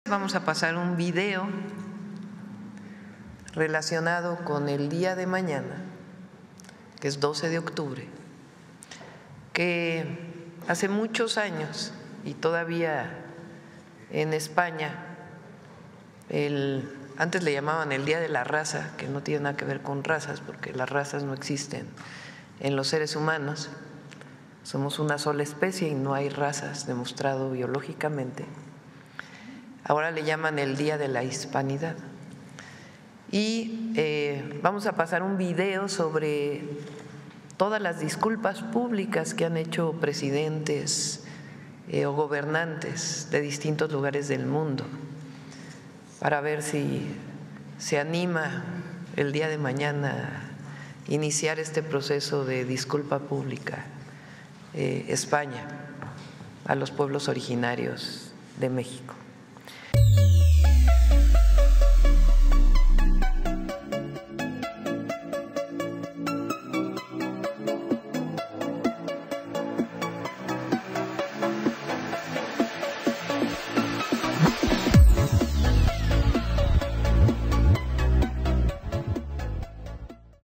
Claudia Sheinbaum, RETA al GOBIERNO de españa, a PEDIR DISCULPAS. | "NO existen las RAZAS".
En un hecho histórico, la Presidenta de México, Claudia Sheinbaum Pardo, lanza un mensaje contundente y directo, al gobierno español.

Saludos...